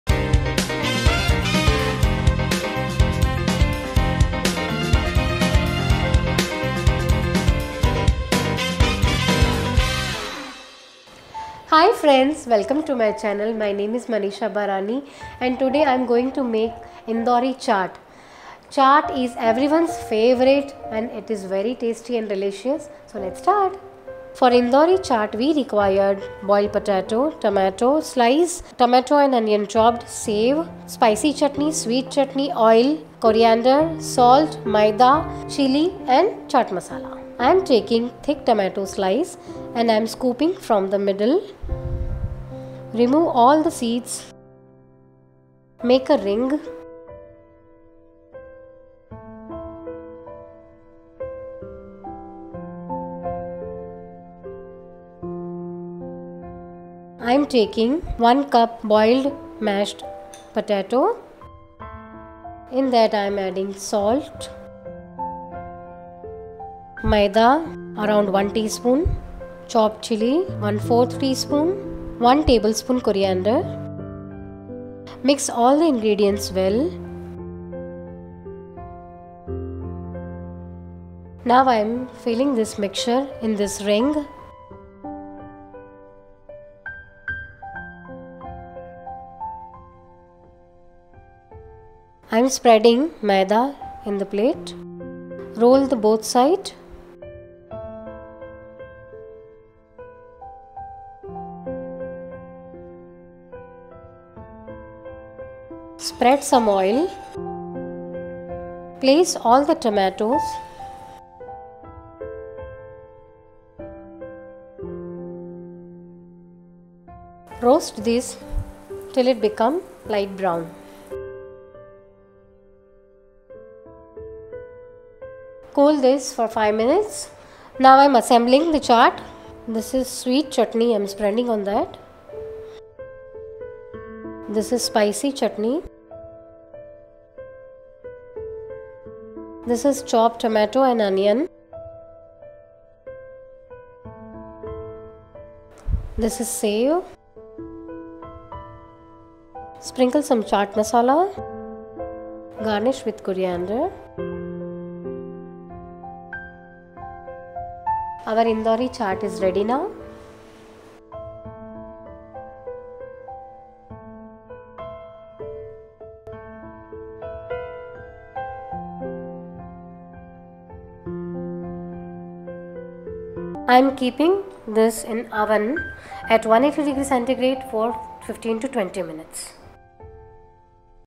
Indori Tamater Chaat - Stuffed Tomato Chaat - Quick Indian Recipe
Indori Tamater Chaat - Stuffed Tomato Chaat - Indian Recipe

INDORI CHAAT-Stuffed Tomato CHAAT is a delicious Indian recipe served as a Snack or Starter.
 Today I would like to introduce you to one of those CHAATS which I have been preparing from long time, giving a real delight to the taste buds.
Stuffed Tomato Slice CHAAT fits perfectly in all the 3 categories. It is nutritious, healthy and a one good for those lengthy chit-chat parties. And the best part is you can make it at home in just a few minutes.
The word CHAAT in Hindi means it’s the name of India's favorite hot & spicy, savory & slightly tangy and a little bit sweet food.

Ingredients : -
4 Tomato Slice(Thick),
1 cup Boiled Potato,
¼ Cup Chopped Tomato & Onion,
4 Tbsp Sweet Chutney,
2 Tbsp Spicy Chutney,
4 Tbsp Thick Sev,
1 tsp Maida,
¼ tsp chopped Green Chillies,
1 Tbsp Coriander,
Salt to Taste. Chaat Masala, Coriander as required.

Procedure : -
Take thick Tomato slice & remove all seeds from the centre.
Make Tomato Rings.
Take boiled Potato in a plate, add chopped Green Chillies, Maida, Salt & Coriander in it & mix well.
Now stuff this Potato mixture inside the Tomato Rings.
Dust some Maida on the both sides of the Tomato Rings.
Spread some oil on a pan & place all Tomato Rings on it.
Roast the Tomato Rings till they become light brown.
Cool it for 5 minutes.
Assemble the CHAAT; first place the roasted stuffed Tomato slice in bowl.
Over this, spread Sweet Chutney, Spicy Chutney, chopped Tomato, Onion, Sev, Chaat Masala, & garnish with Coriander.